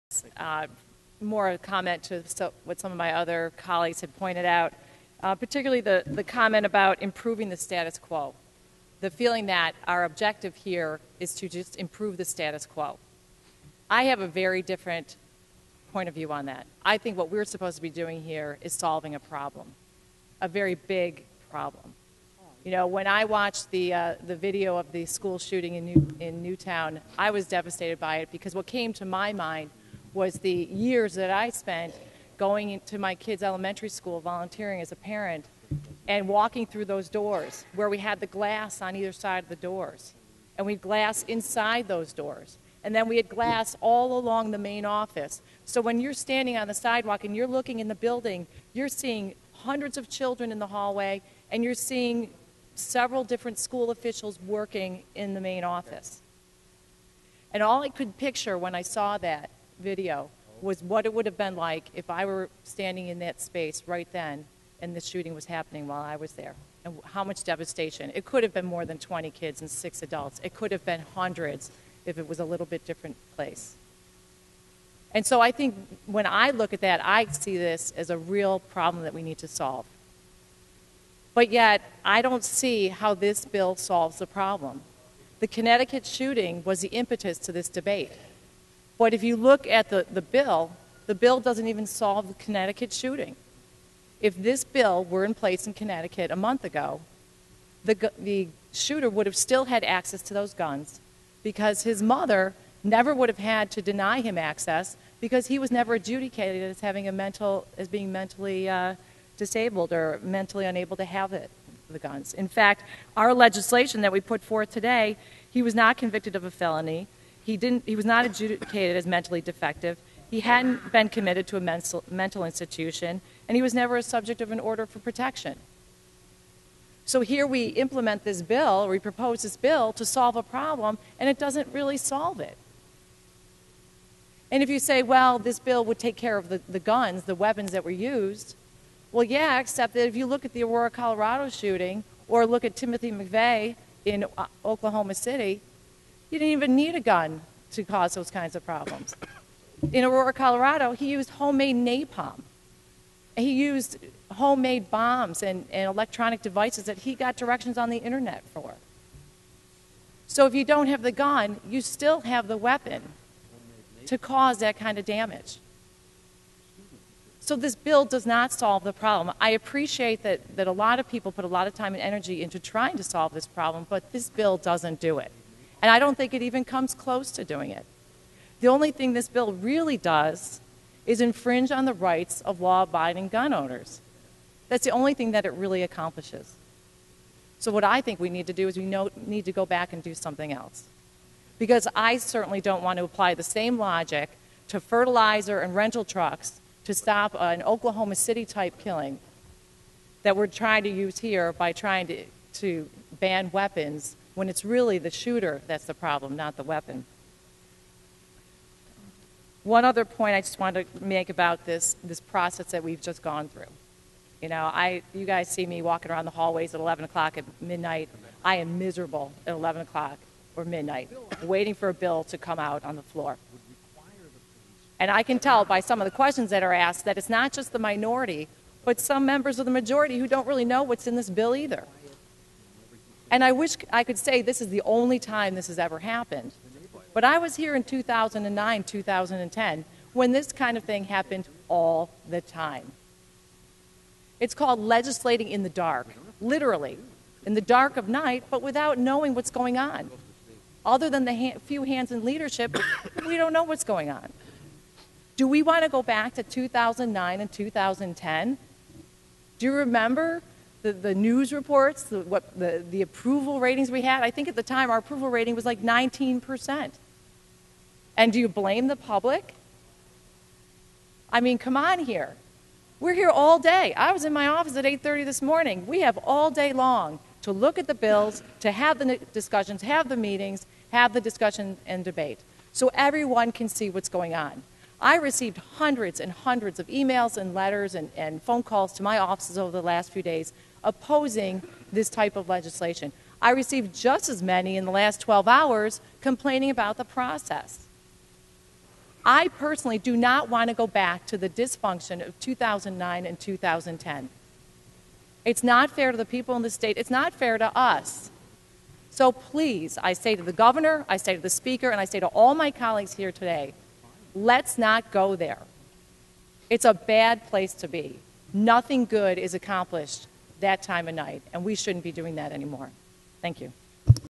NYS Assemblywoman Jane Corwin Stands Up for Second Amendment Rights
On January 15, 2013, NYS Assemblywoman Jane Corwin spoke against legislation that would infringe on law-abiding New Yorkers' Second Amendment Rights. To learn more about the Assemblywoman's views, please see her